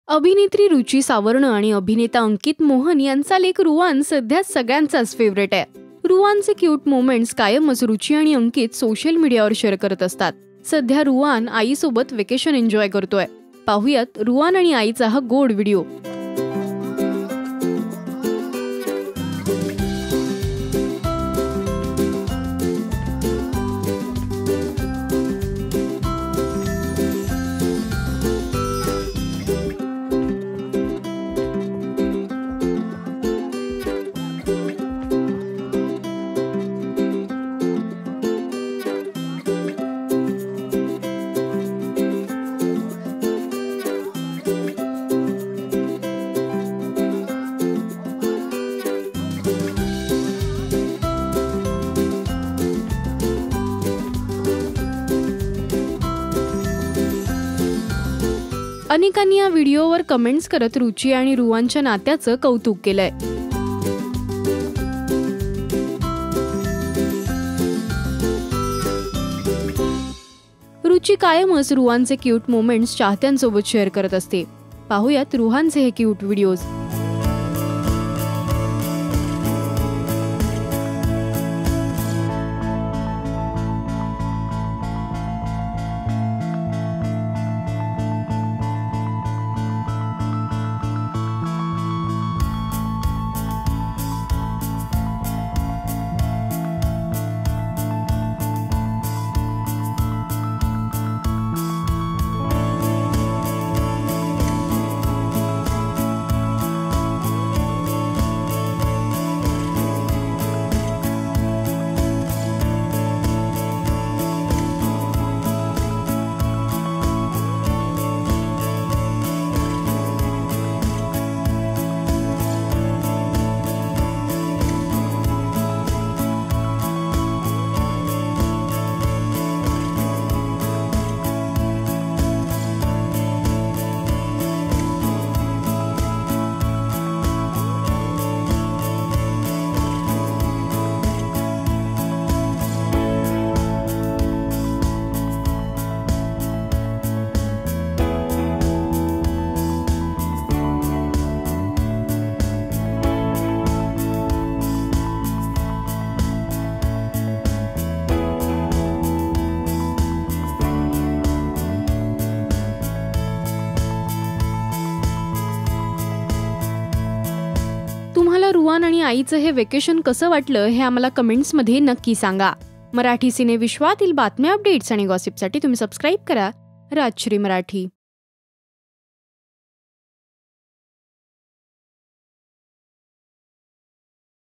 Ruan Enjoying Vacation With His Mother Ruchi Savarn | मायलेकाचं धमाल VACATION | Rajshri Marathi
अभिनेत्री रुची सावर्ण लेक रुआनसोबत VACATION एन्जॉय करतेय. पाहुयात आई  आणि रुआनचा हा खास व्हिडीओ.